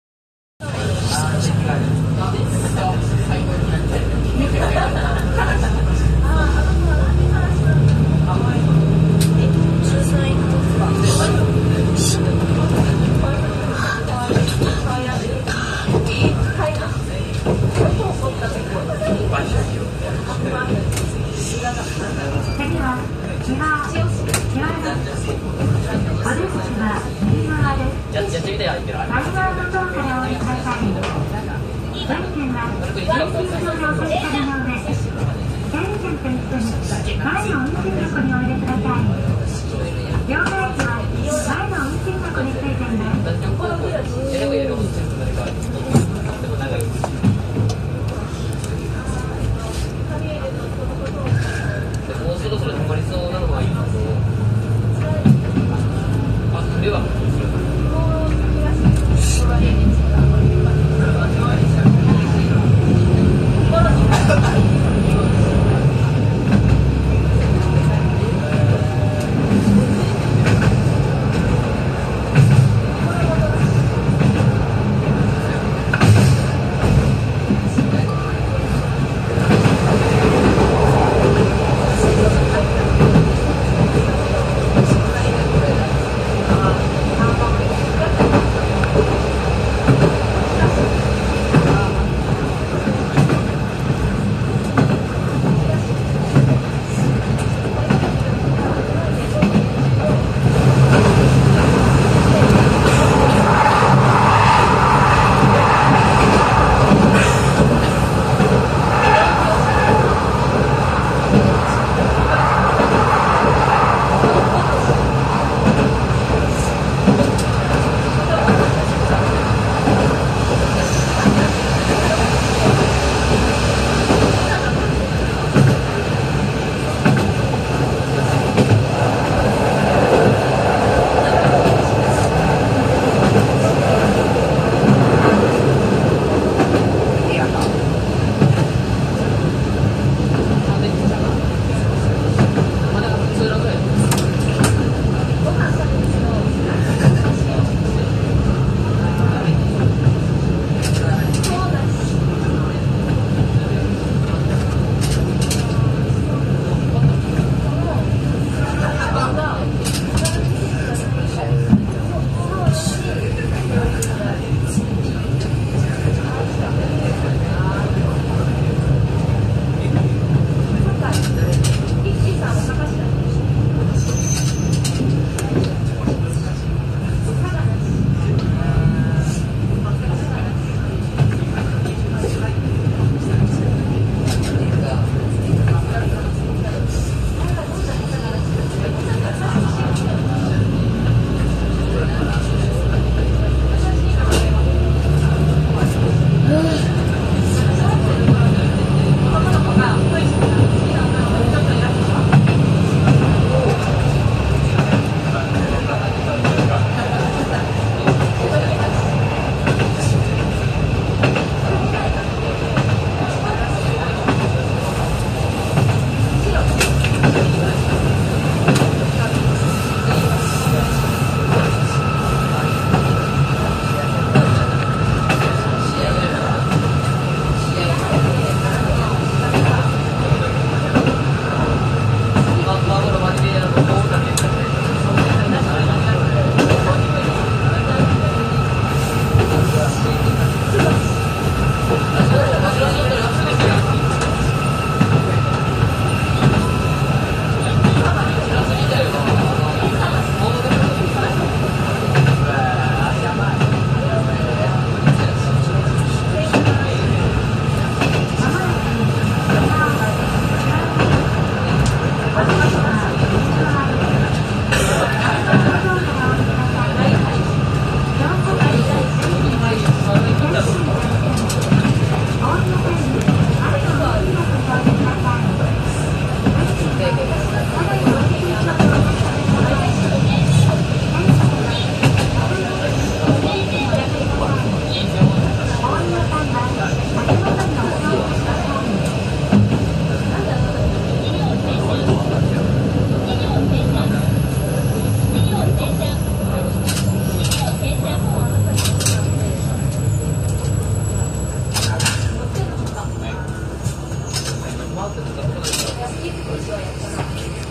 【前面展望】迫力満点！因美線 美作河井駅ー知和駅
超ローカル線の因美線美作河井駅から秘境駅の知和駅までの前面展望です。
鉄橋、トンネルといろんな情景が楽しめますよ。